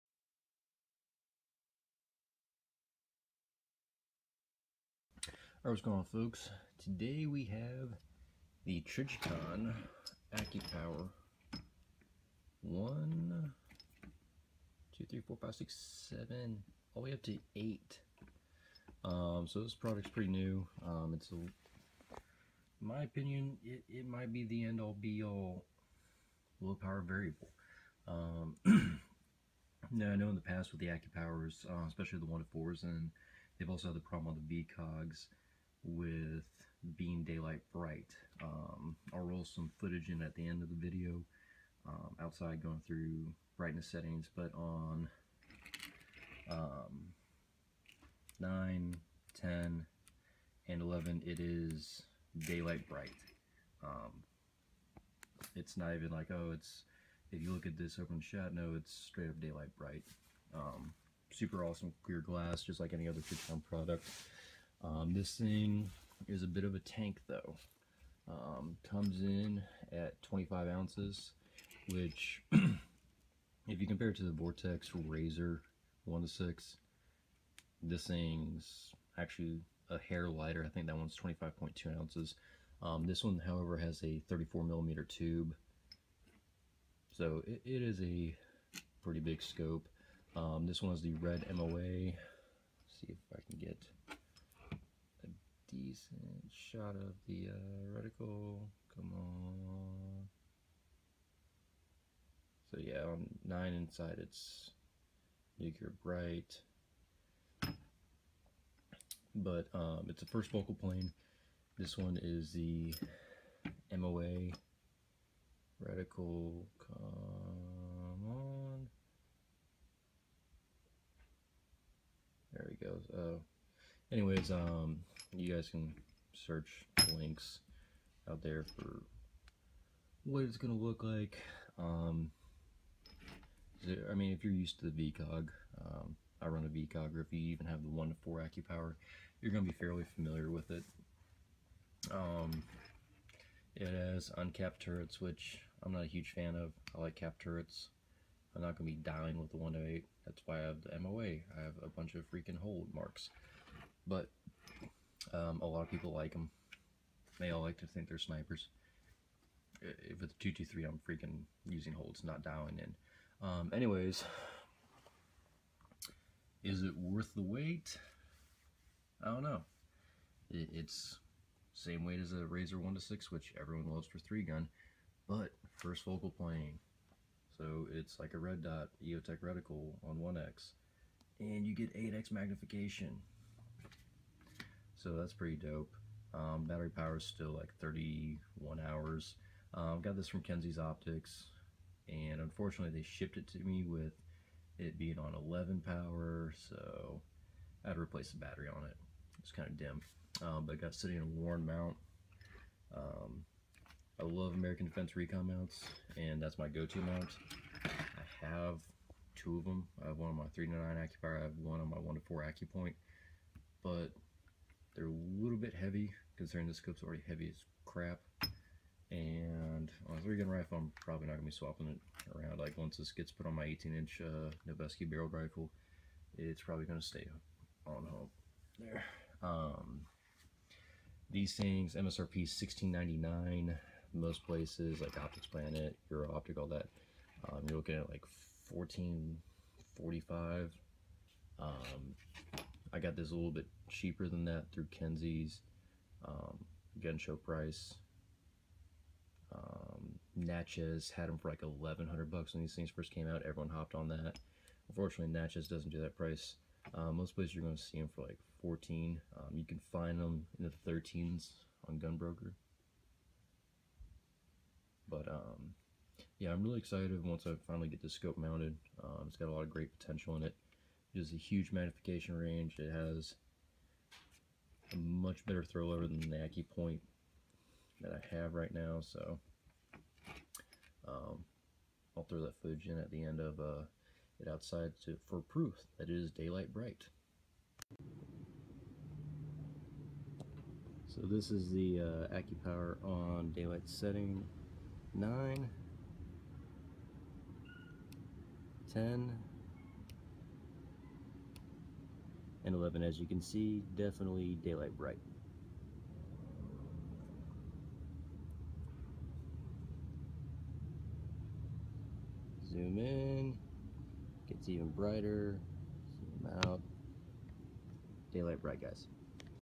Trijicon Accupower 1-8 Initial Impressions
This may be the best 3gun scope out there. The red MOA is absolutely daylight bright, it has better glass than vortex, made in america, 1/4 MOA adjustments and it's a freaking 1-8